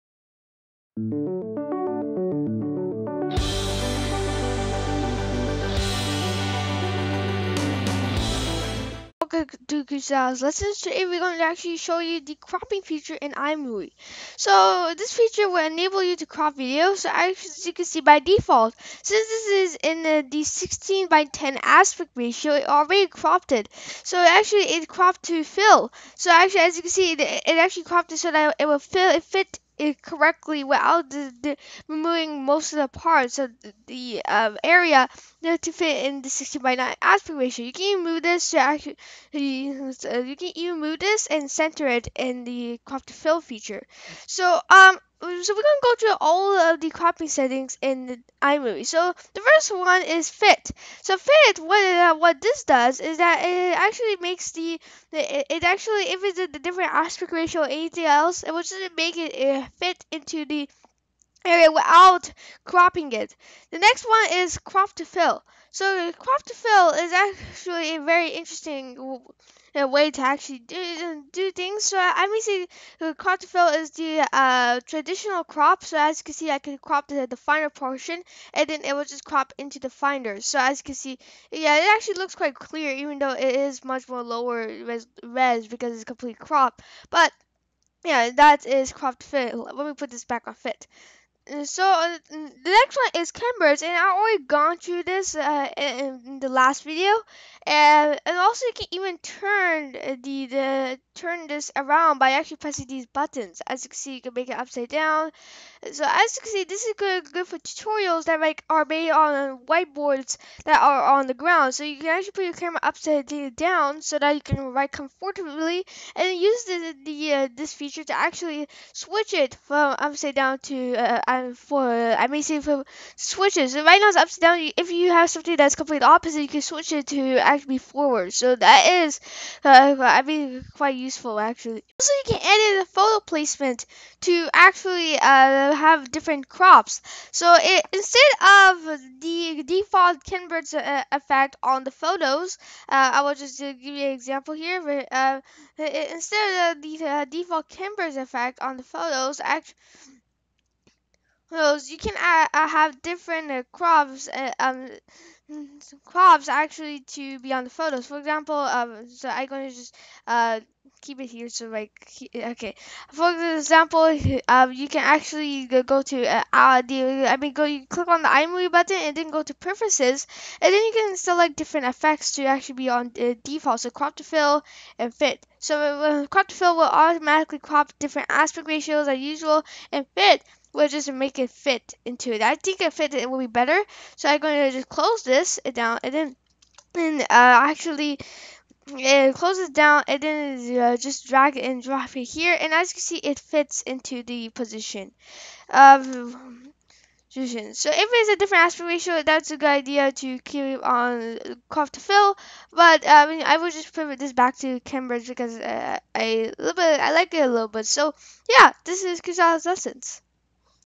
iMovie 2016 Cropping
This video would show you how to crop in iMovie 2016.